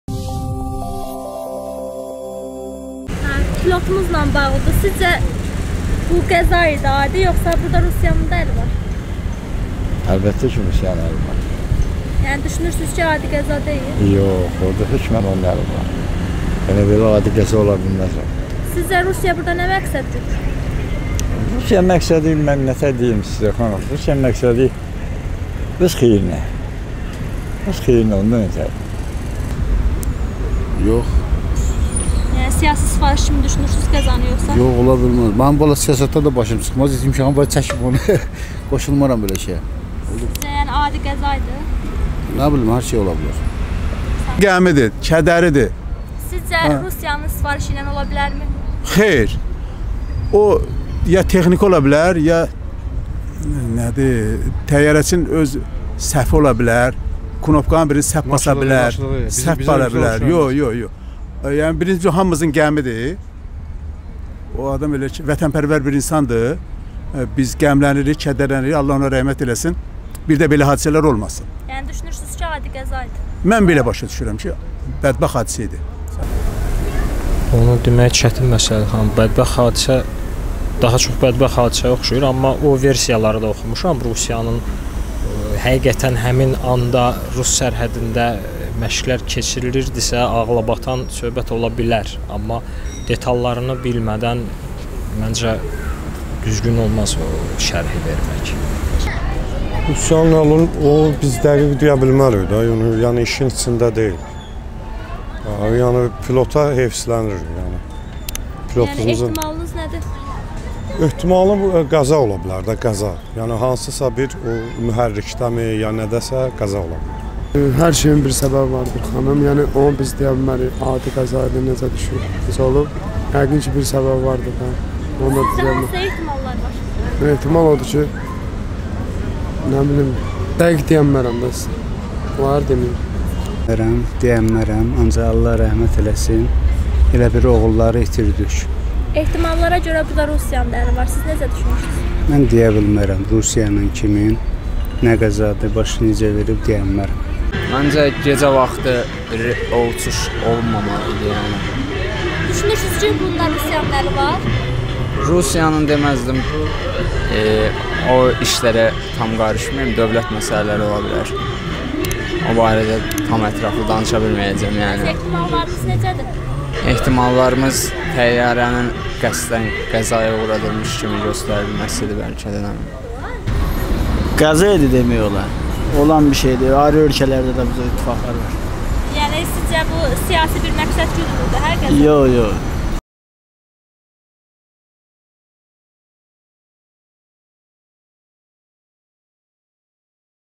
"Bu işdə rusun əli var, bu adi qəza ola bilməz"-Bakı sakinləri təyyarə qəzası barədə
"Yəqin pilotun səhvidir, təxribat ola bilməz"-Pilotun qəzaya uğramasında Rusiyanın əli varmı?-SORĞU